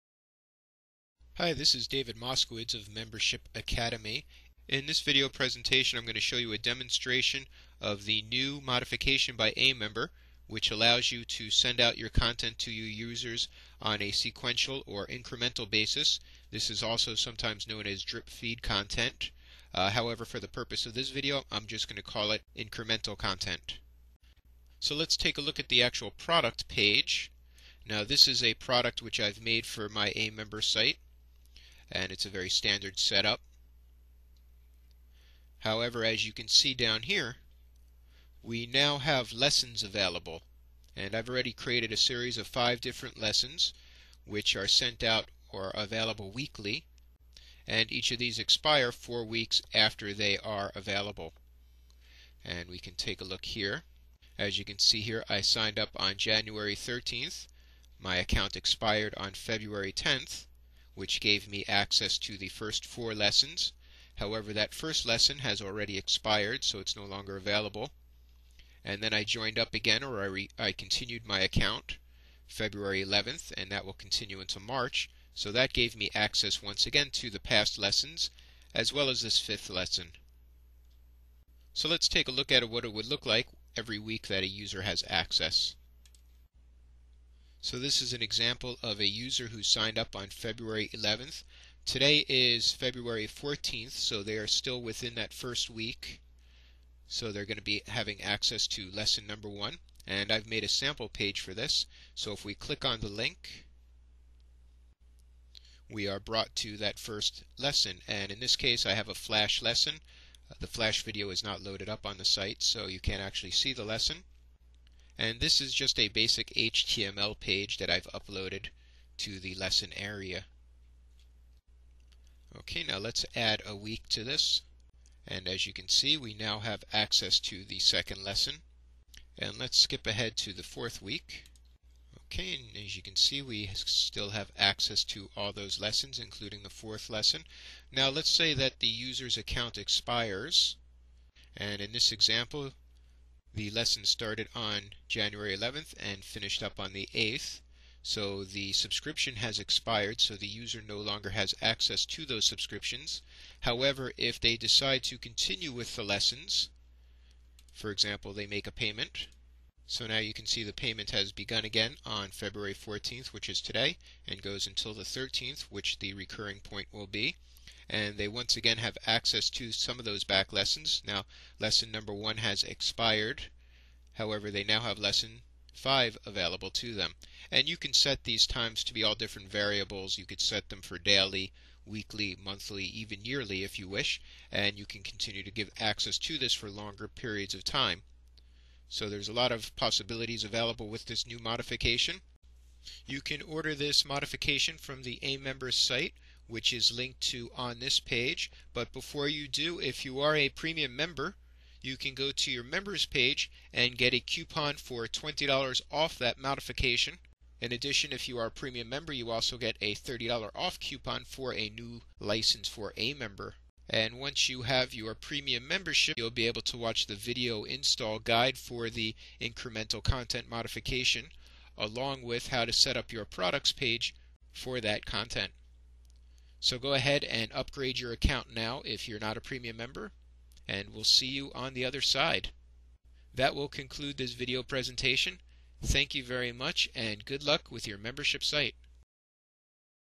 Incremental content on your membership site demonstration.
Watch the video demonstration of the new incremental content plugin from aMember. This will allow you to drip feed content to your members.